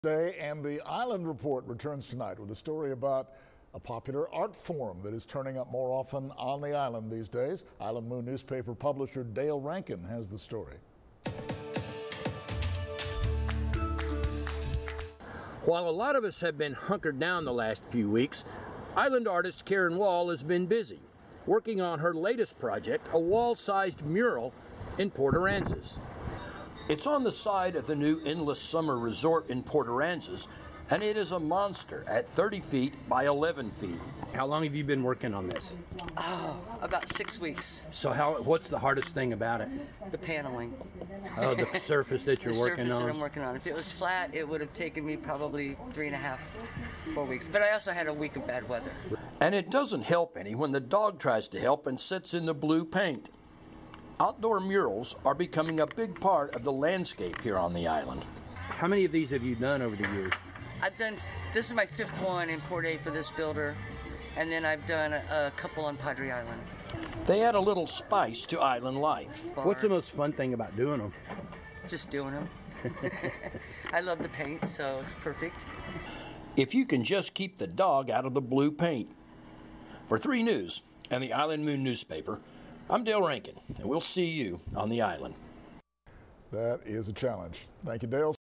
Island Report: Port Aransas Mural
A popular art form is turning up more often on the island these days. Island Moon Newspaper publisher Dale Rankin has the story.